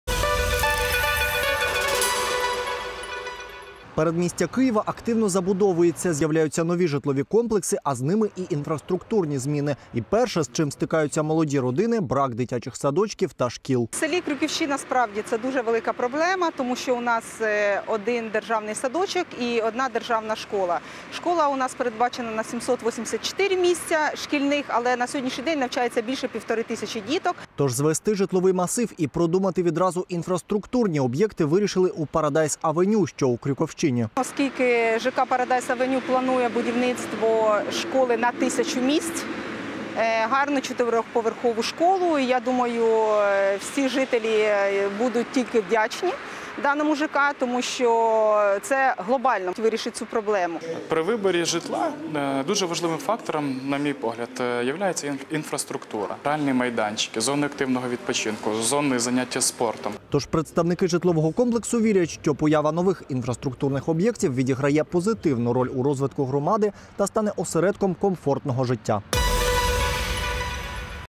Развитие общества и центр комфорта: "Парадайс авеню" о своем современном жилом массиве
Построить жилой массив и продумать сразу инфраструктурные объекты решили в «Парадайс авеню», что в Крюковщине.